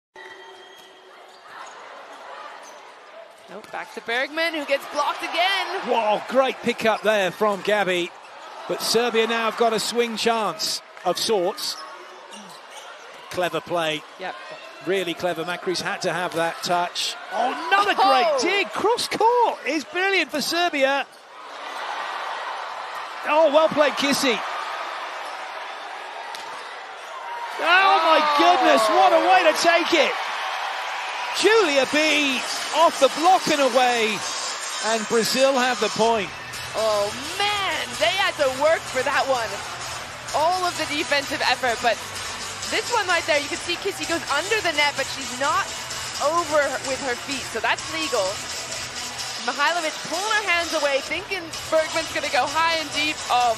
Brasil bate Sérvia e decide título inédito contra Itália na Liga
Comunicação e informativo cA seleção feminina brasileira de vôlei está a uma vitória da conquista do título inédito na Liga das Nações, depois que venceu a Sérvia de virada neste sábado (16), por 3 sets a 1, em Ancara (Turquia), com parciais de 14/25, 25/18, 26/24 e 25/19. A decisão do título será contra a Itália, às 12h30 (horário de Brasília) deste domingo (17) - as italianas derrotaram a Turquia, por 3 sets a 0, na outra semi deste sábado (16). Será a terceira disputa de título do Brasil na competição: a seleção feminina bateu na trave em 2019 e 2021, ficando com o vice-campeonato. om transparêcia e seriedade.